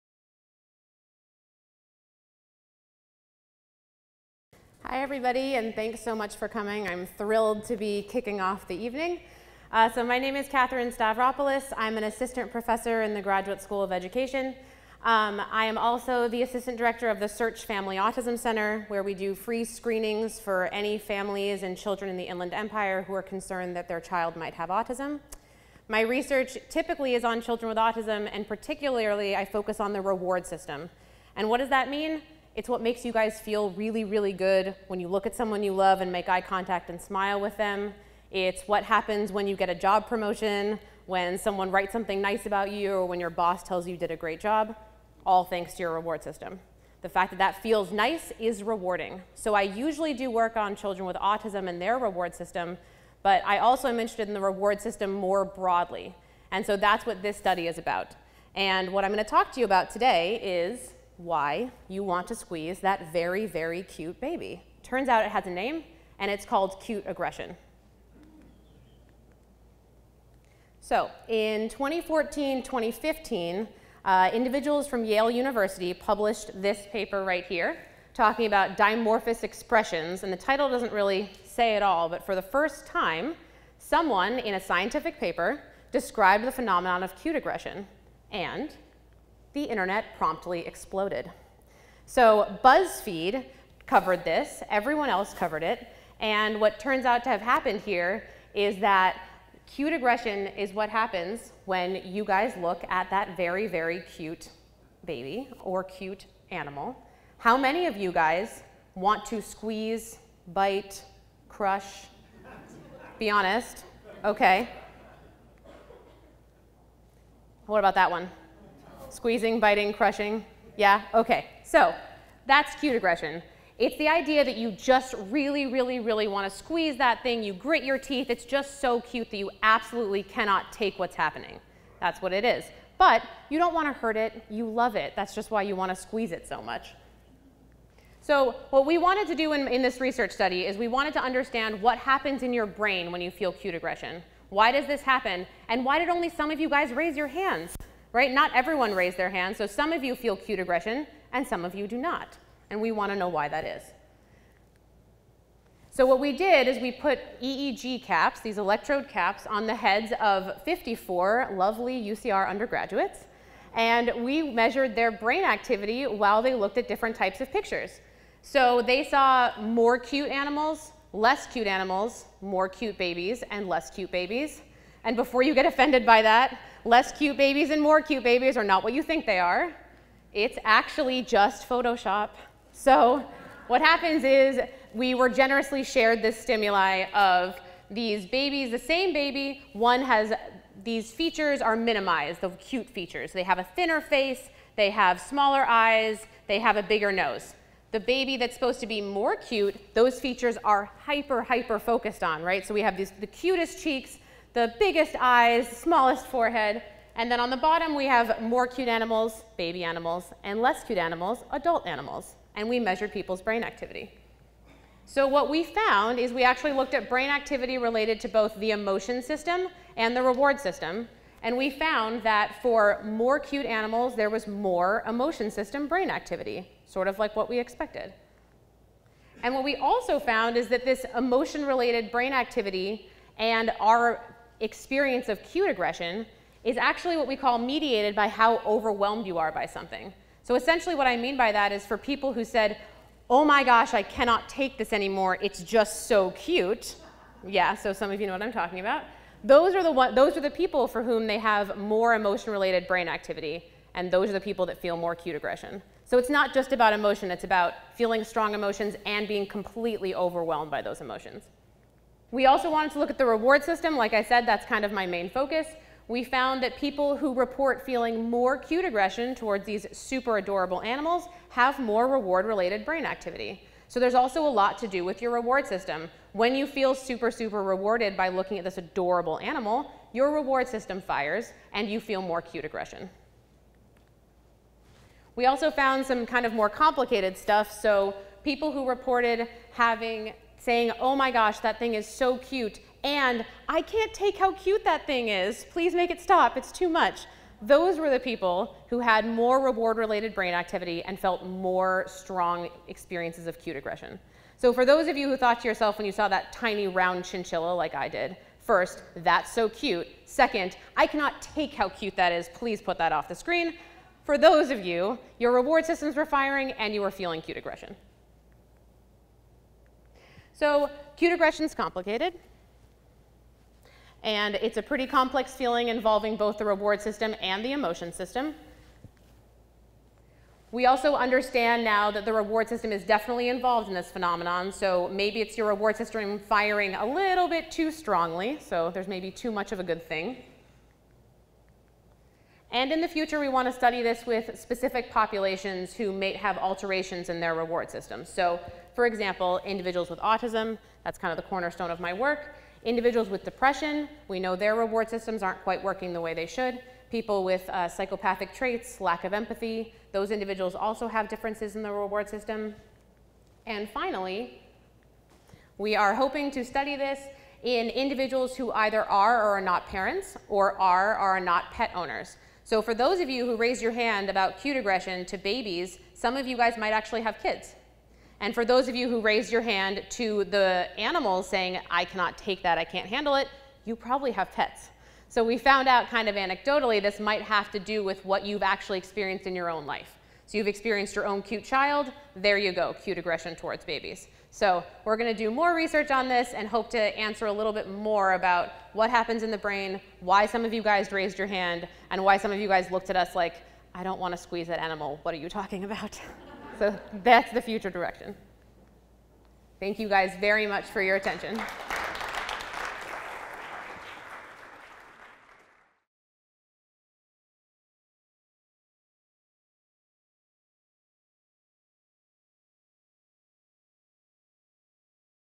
Why Do You Want to Squeeze That Adorable Baby? Cute Aggression and the Brain
Have you ever looked at a puppy and had the urge to squeeze or even bite it? Or felt compelled to pinch a baby’s cheeks, albeit without a desire to harm it? Dr. Katherine Stavropoulos, assistant professor at the Graduate School of Education, helps us understand what happens to the brain when individuals experience this phenomenon called “cute aggression.”